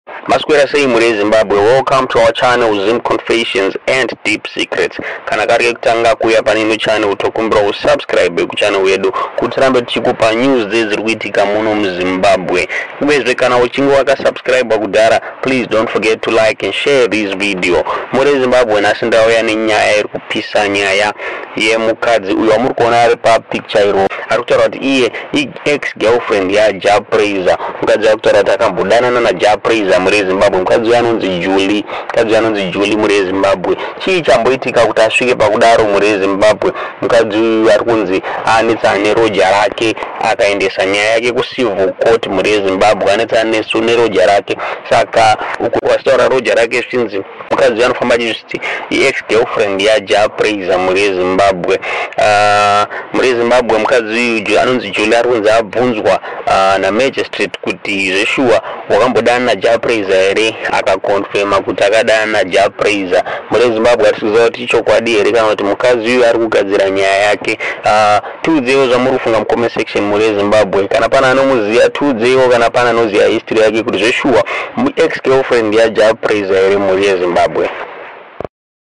Mukadzi Uyu Arikuti Akadanana Na Jah Prayzah Vakazorambana
Mukadzi Uyu Arikuzviti Akadanana Na Jah Prayzah